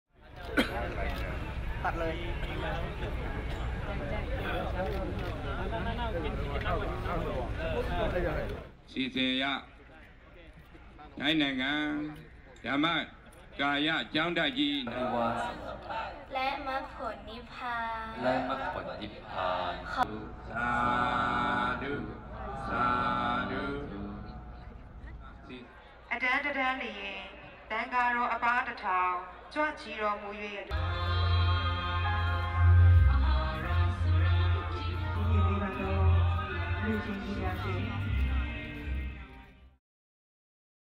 30.000 Mönche bitten um Spenden | AFP
In Myanmar haben sich mehr als 30.000 buddhistische Mönche getroffen, um gemeinsam um Spenden zu werben.